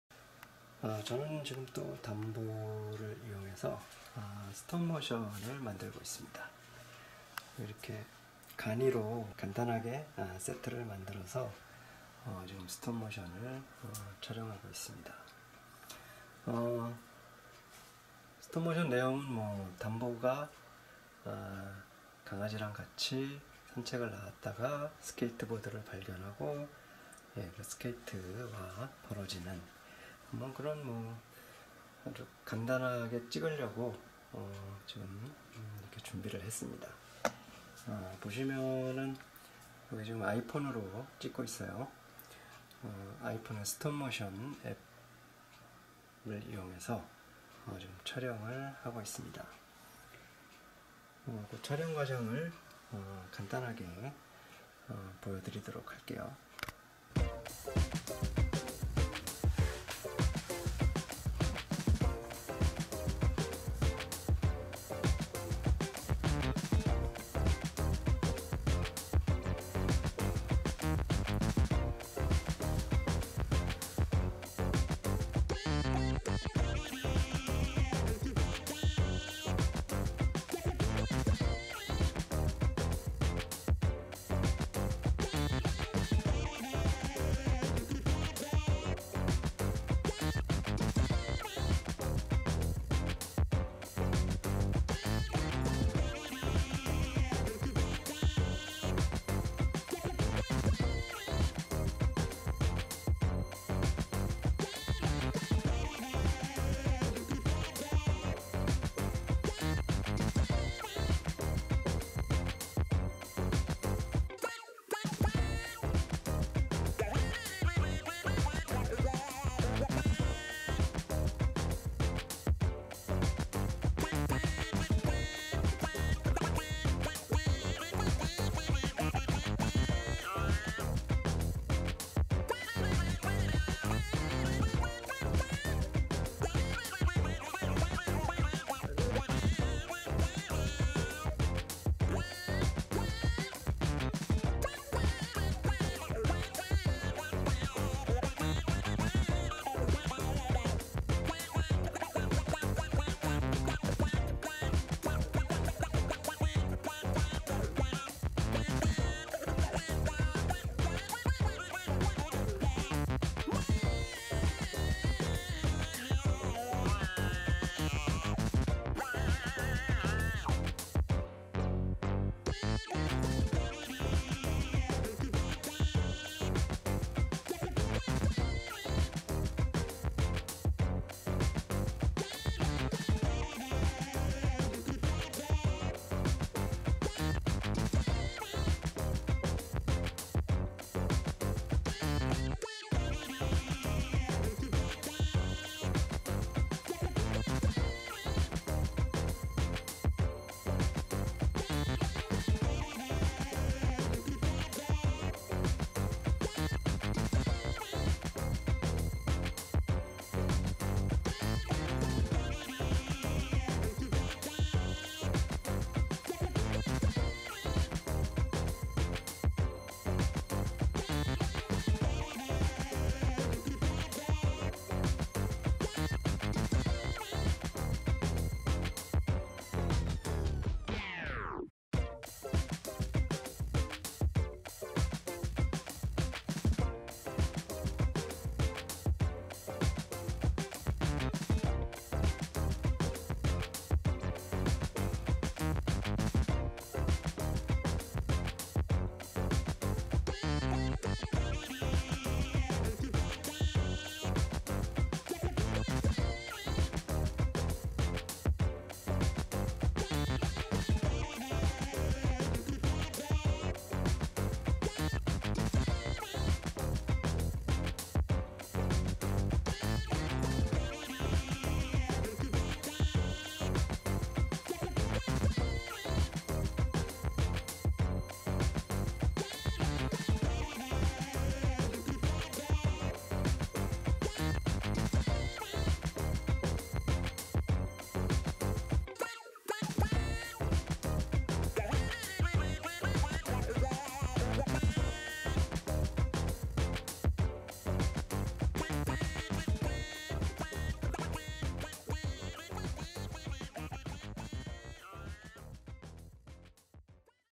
iPone Stop Motion 'Danbo & Skateboard' Making Video 아이폰 스톱모션 메이킹 영상 '단보와 스케이트보드'
아이폰 스톱모션 앱으로 촬영한 스톱모션 - 단보와 스케이트보드의 짤막한 메이킹 영상입니다~